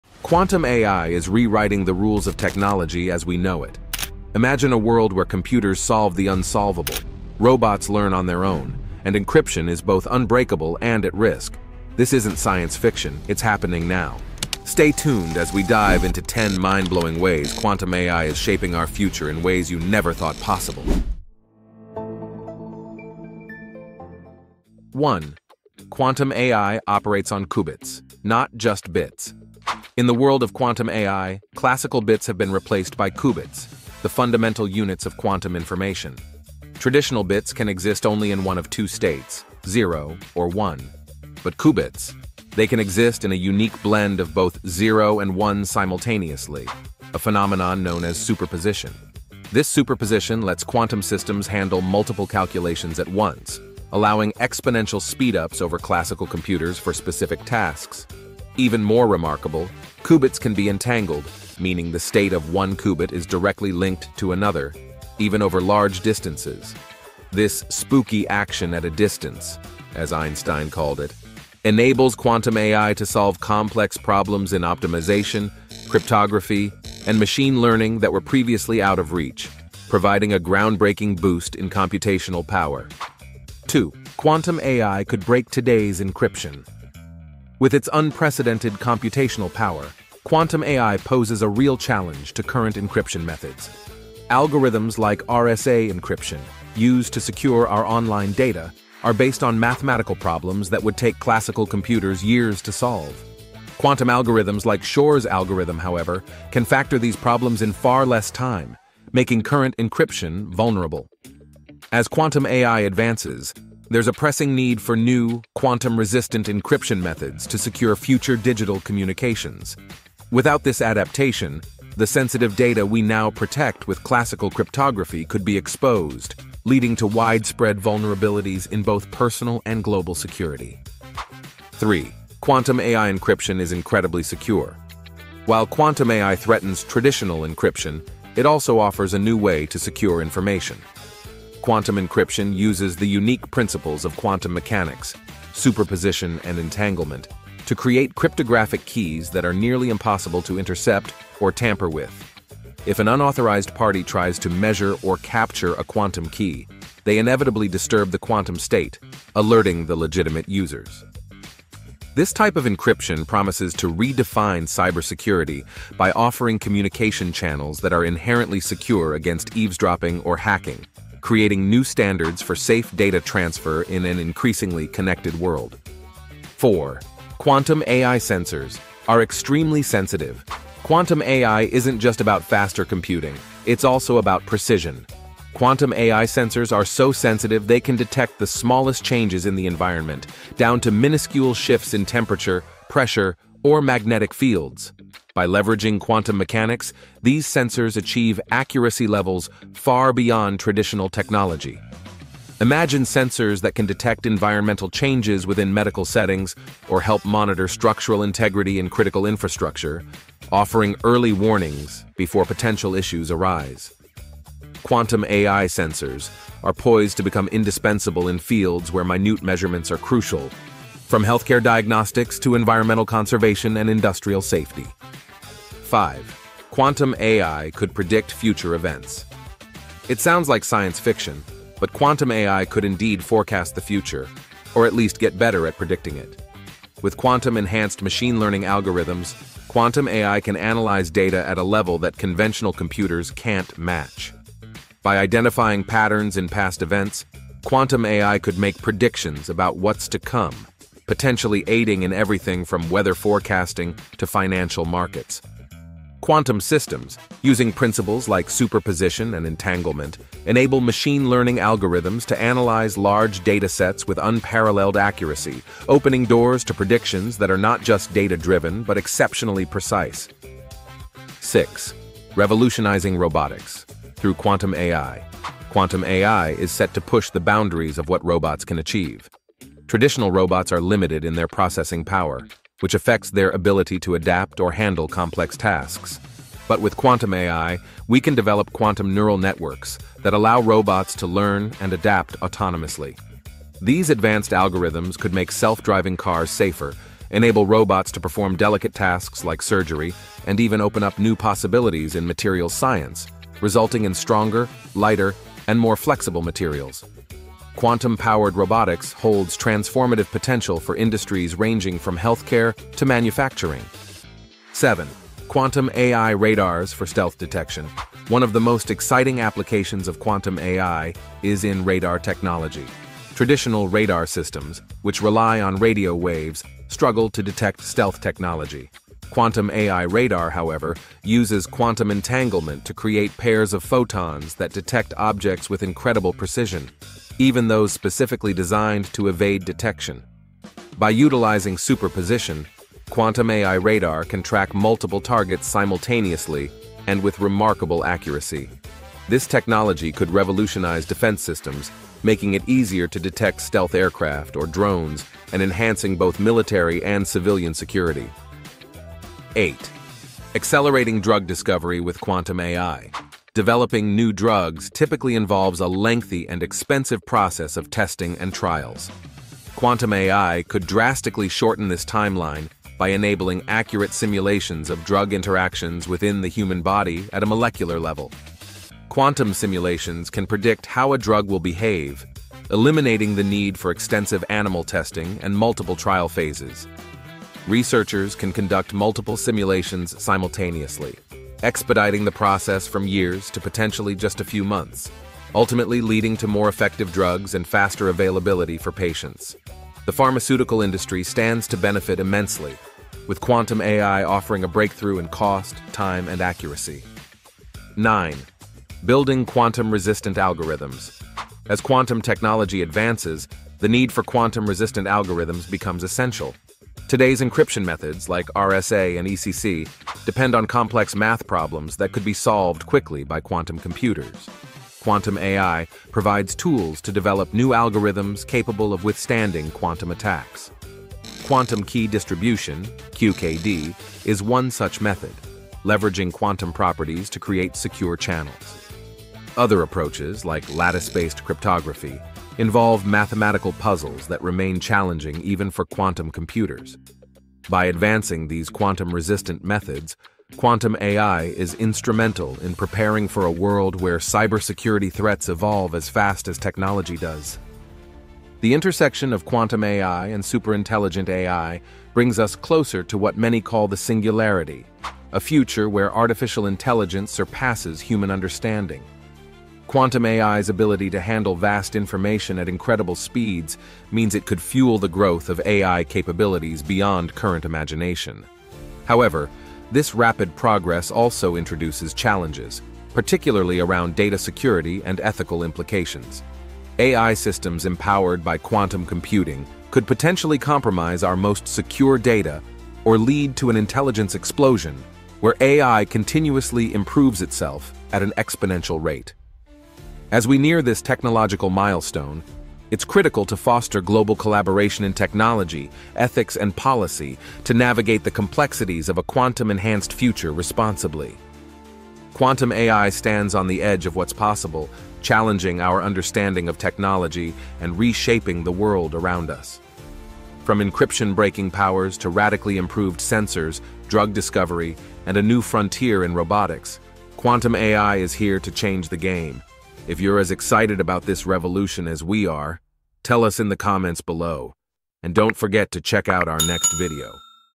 Quantum AI Revolution Will CHANGE EVERYTHING!
Experience the future with Quantum AI! Learn how this groundbreaking technology will revolutionize industries and change the way we think about AI.